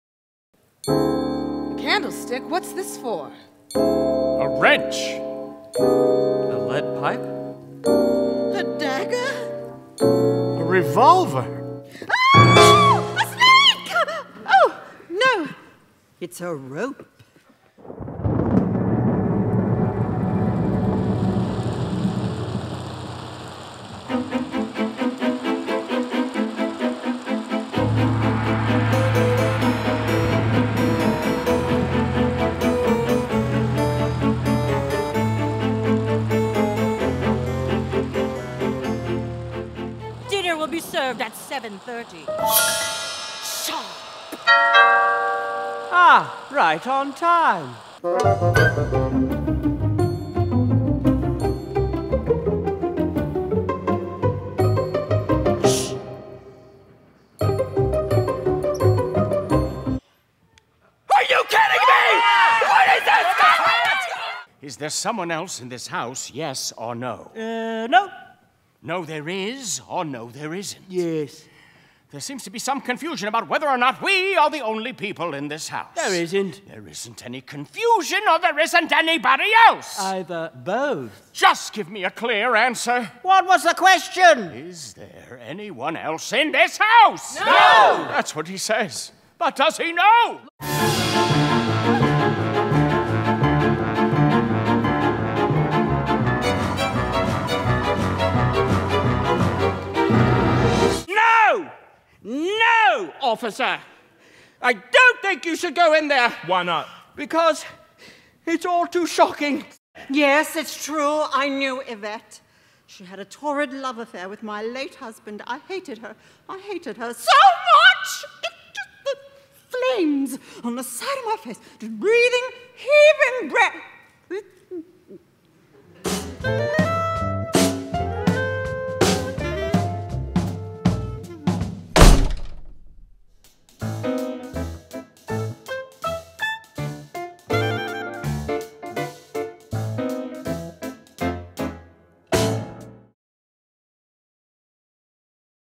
CLUE | Paper Mill Playhouse
Murder and blackmail are on the menu when six mysterious guests assemble for a dinner party at Boddy Manor. Was it Mrs. Peacock in the study with the knife? Or Colonel Mustard in the library with the wrench? Based on the cult 1985 Paramount movie and inspired by the classic Hasbro Bros board game, this farce-meets-murder-mystery is bloody good fun!